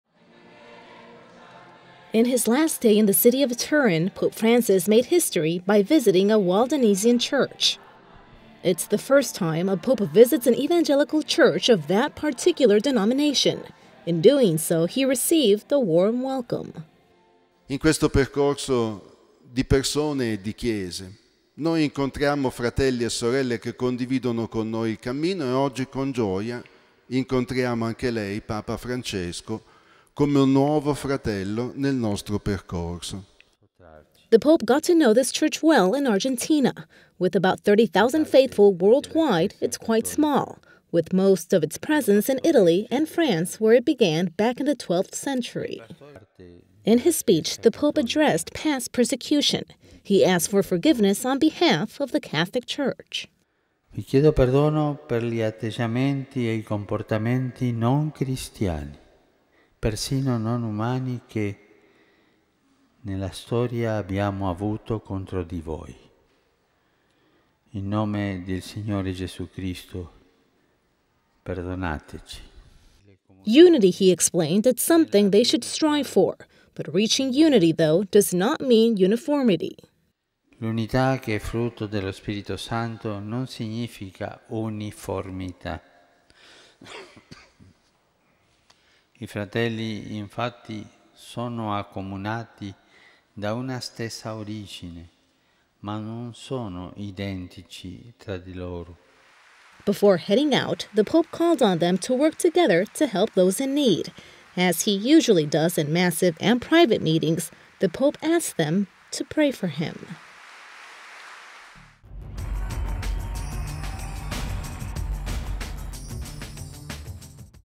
Pope to Waldensian Church: Forgive us!
The evangelical denomination only has about 30,000 members worldwide.